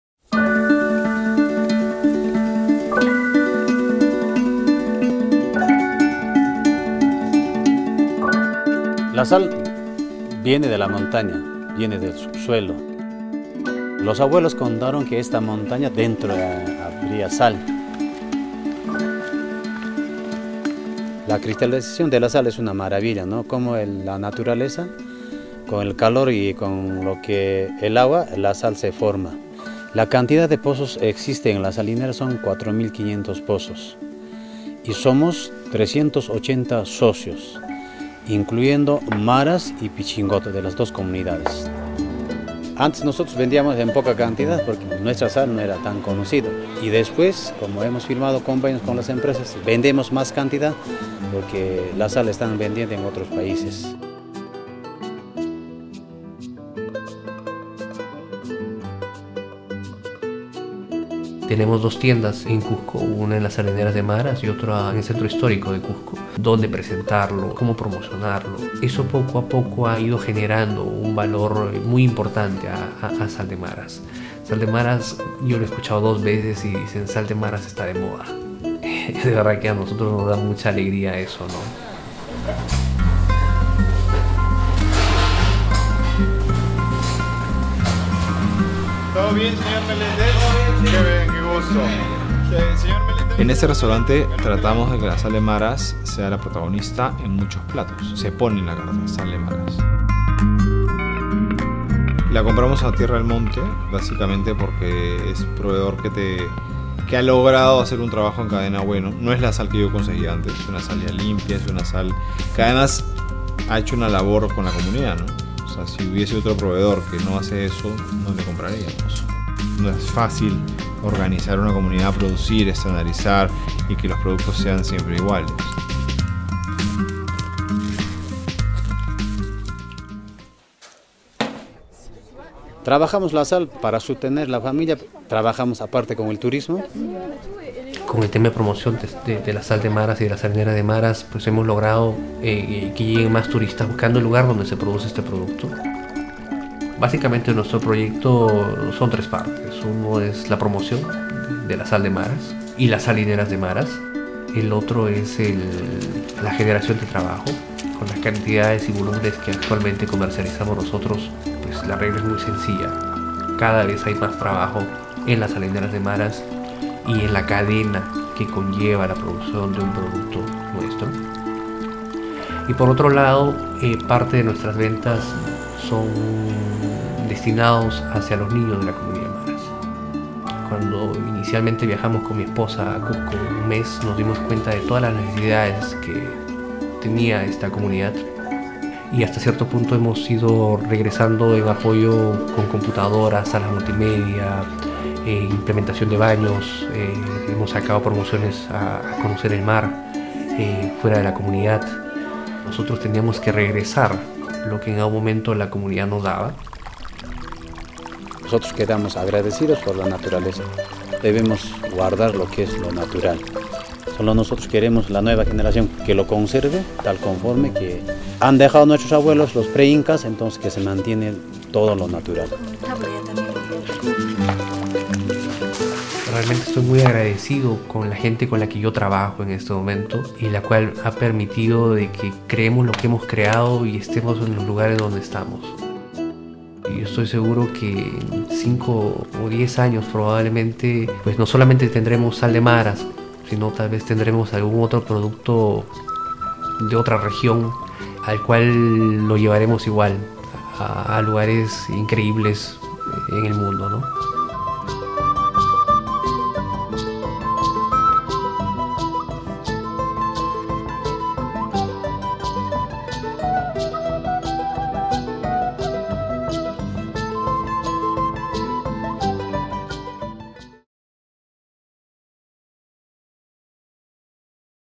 Sal de Maras (Cusco) / Maras Pink Salt (Cuzco)
Find out more about how Maras or Cuzco pink salt is produced, where it comes from and the communities involved. This salt is also found in some Pacari Chocolates bars. You can try and get from: